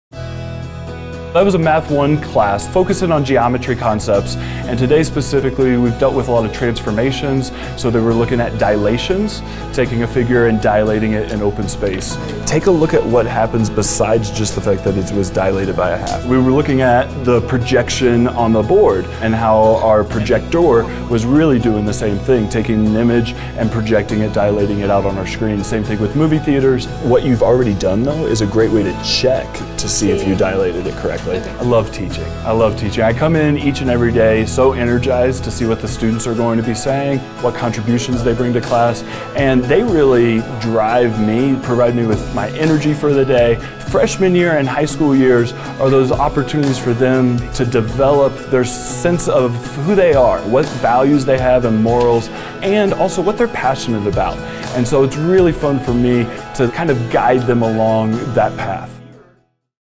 Colorado Academy Upper School Math Teacher, Paul Augustus, on 'Why I Teach'
to learn more about supporting The CA Fund.